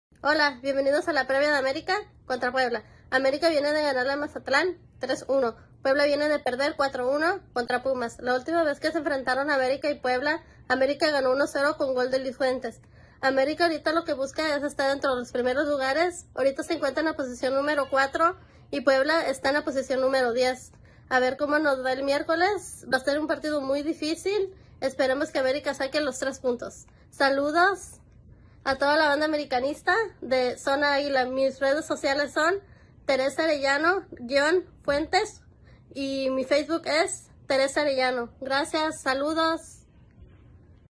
nos da la Previa de cara a nuestro siguente partido
@terearellano_fuentes nos da la Previa de cara a nuestro siguente partido ante el @clubpuebla TeamZonaAguila🦅